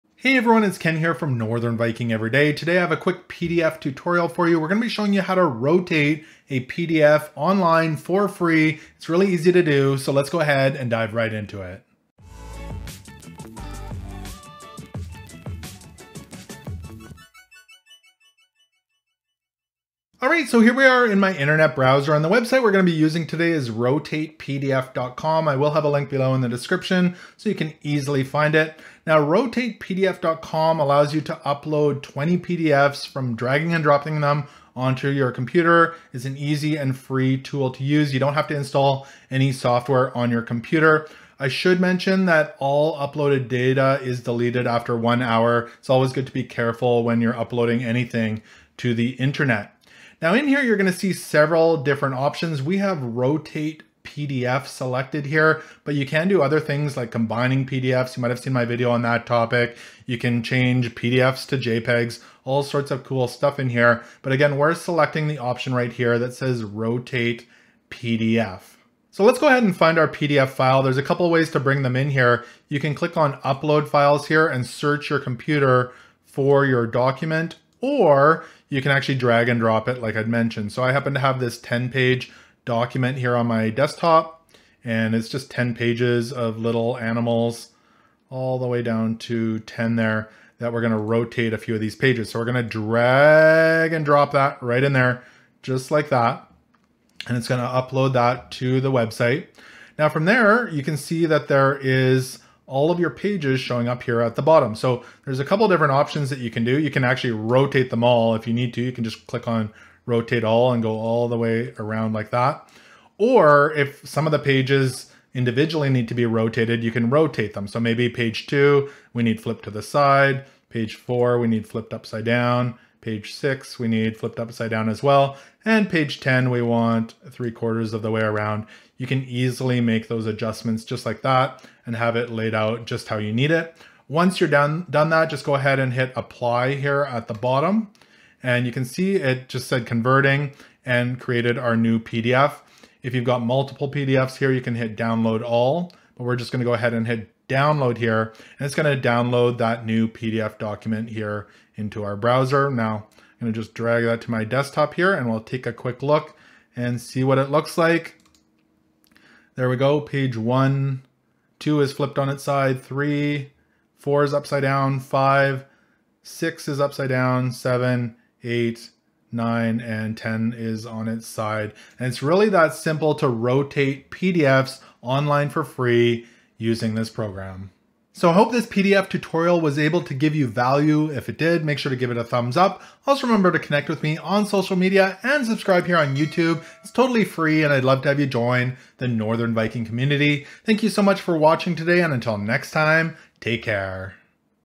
How to Rotate PDF Online for Free
Are you wondering how to rotate the pages in your PDF file without Acrobat? In this tutorial, I will show you how to rotate a PDF online for Free using rotatepdf. This web-based PDF editor will have the pages of your PDF document rotated however you need them in a few simple steps.